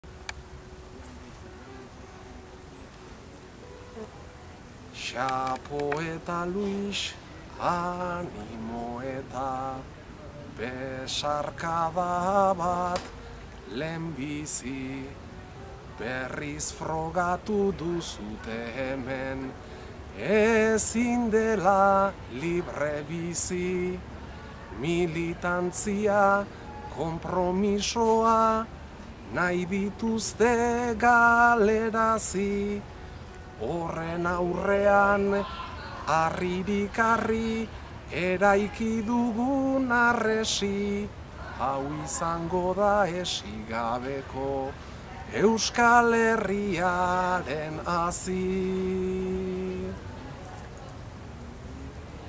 HerriHarresia Xabier Terrerosen elkartasun mezua
Luis Goñi eta Xabier Sagardoi "Xapo"-ren mezua daraman harria eskuz esku, herriz herri dihoa Euskal Herritik zehar. Oraingo honetan, Xabier Terreros bertsolariak jaso du eta bere mezua plazaratu du. Urriak 5, eguerdiko 12etan Golem-etik, manifestazioa izango da "Desobedientziaz libre" lelopean.